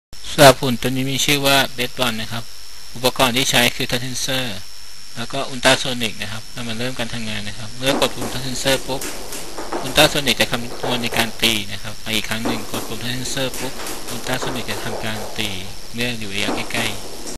ICT SCHOOL ROBOTICS BASE BALL
วัตถุประสงค์
1. เรียนรู้ระยะการตีของ ไม้ Baseball โดยใช้ Touch Sensor และ Ultrasonic Sensor
1. Touch Sensor 1 ตัว
2. Ultrasonic Sensor 1 ตัว